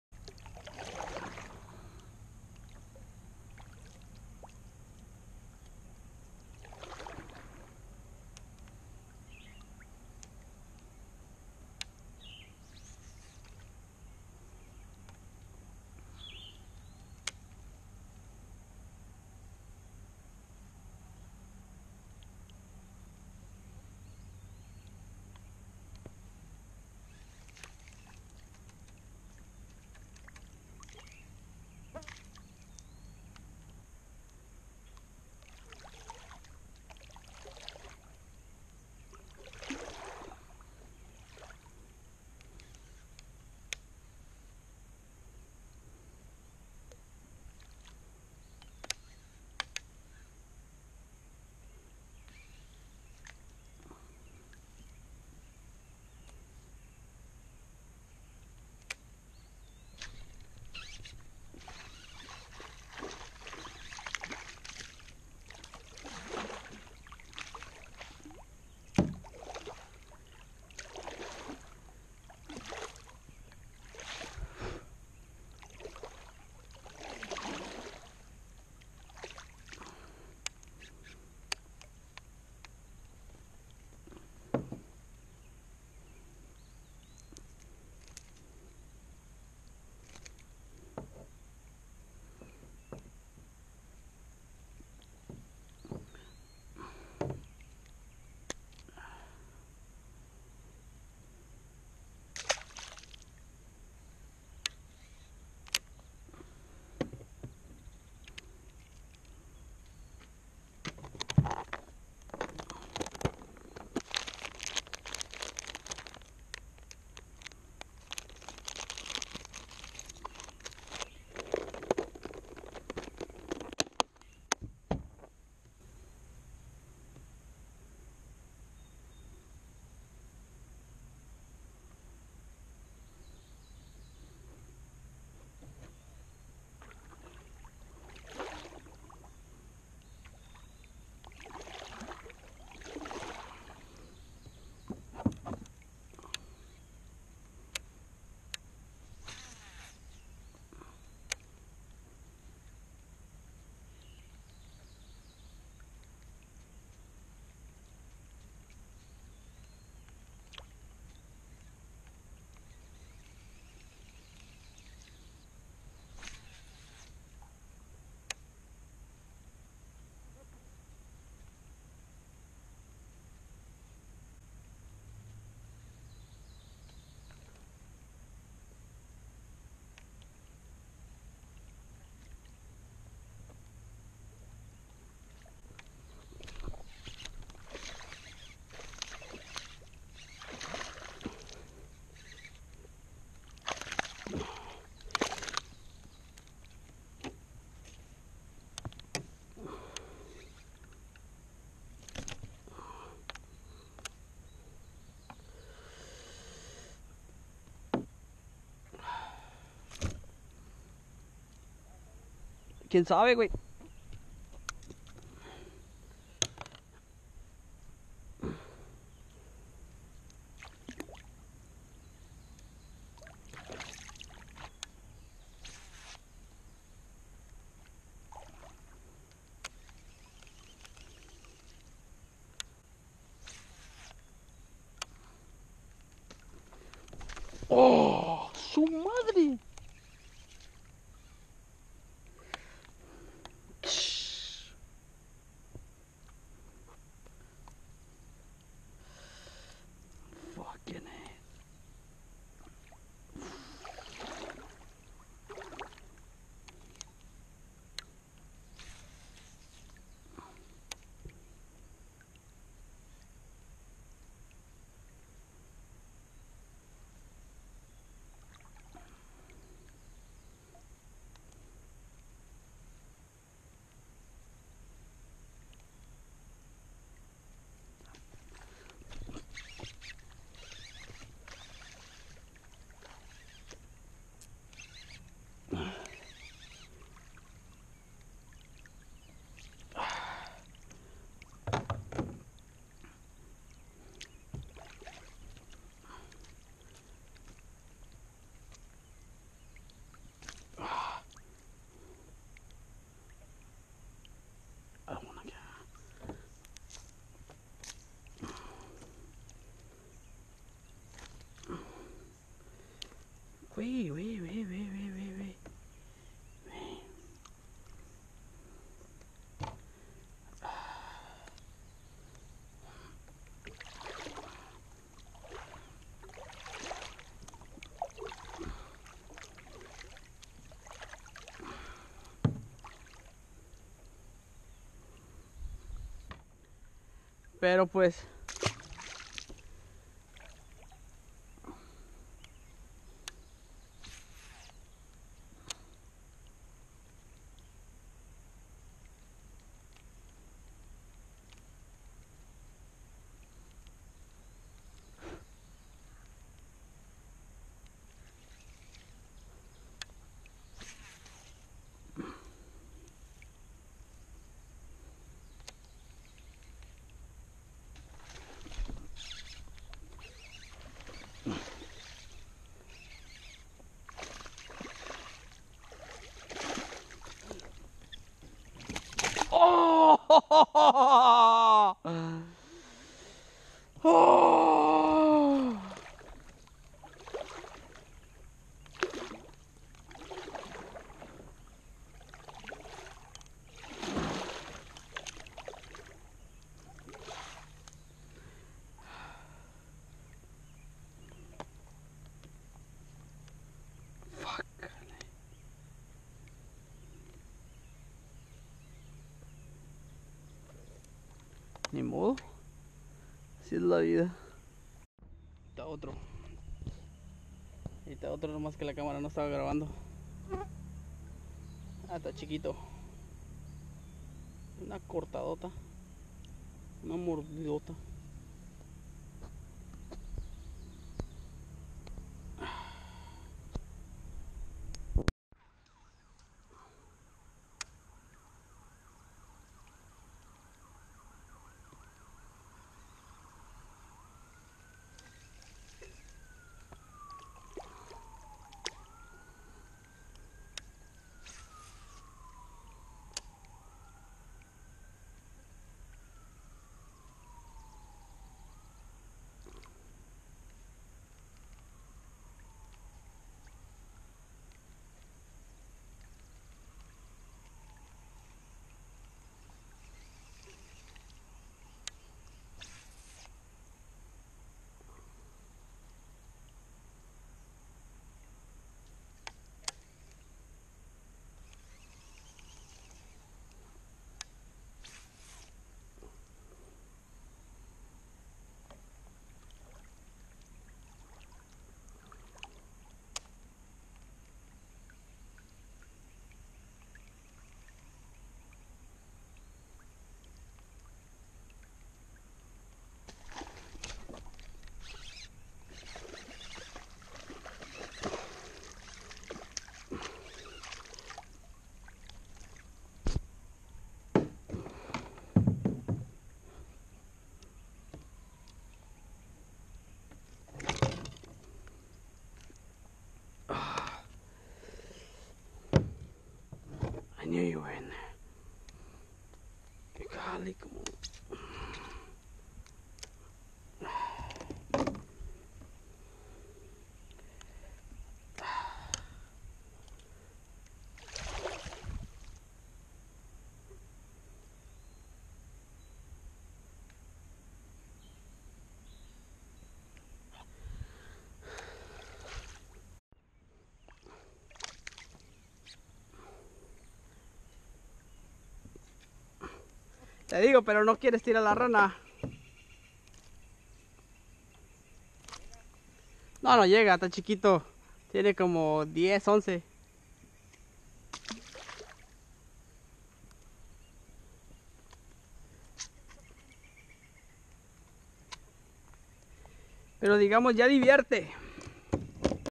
pescando LOBINAS con RANA en KAYAK
Pescando lobinas con mi siñuelo favorito en kayak por la mañana